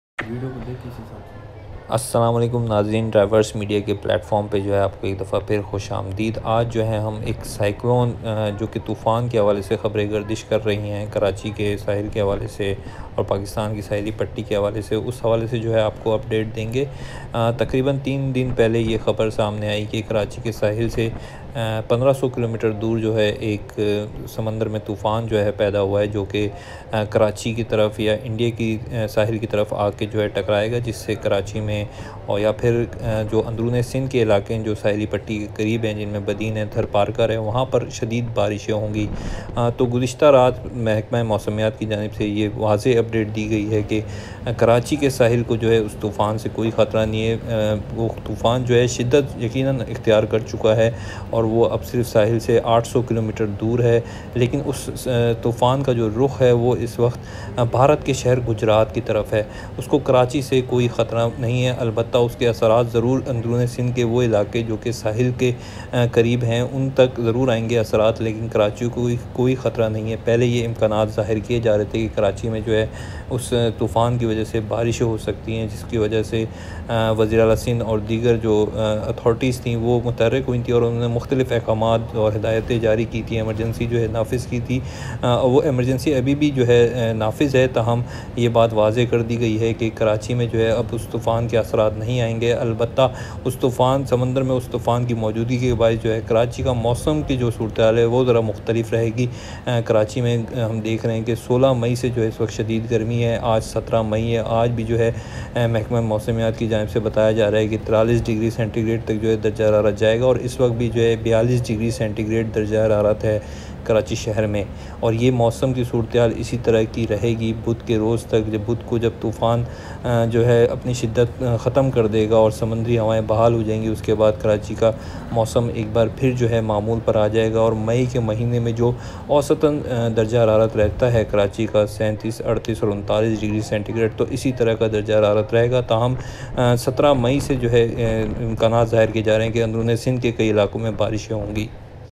Tauktae Cyclone | Karachi Sea | Storm in Karachi
Tauktae storm is about to hit Indian sea!